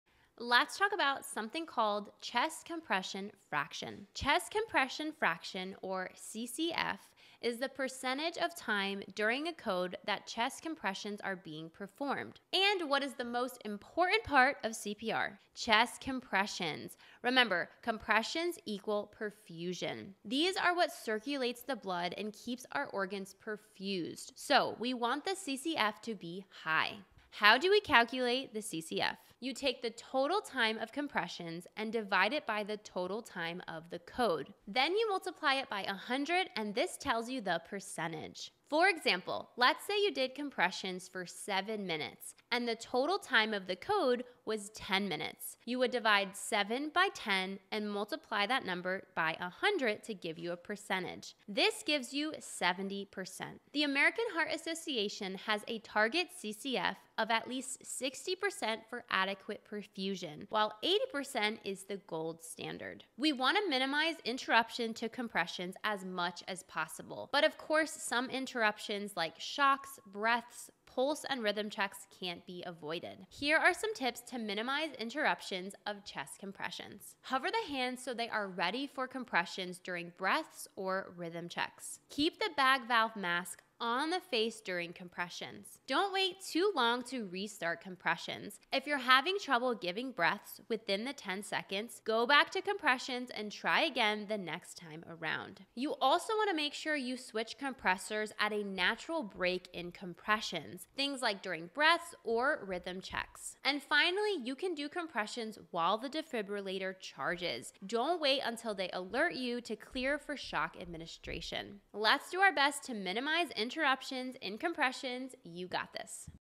Chest Compression Fraction
🚨 CCF = Chest compression fraction

CCF = % of time doing compressions during a code
CCF = total time doing compressions/total time of code, and multiply by 100

We want to be doing compressions as much as possible during a code. This is what moves the blood to our organs! Remember, we must be over 60% but hoping to be closer to 80%.

Here’s another practice problem for you. Let’s say you had a 15-minute-long code and the team was doing compressions for a total of 11 minutes. What would the chest compression fraction be? Put your answer in the comments below!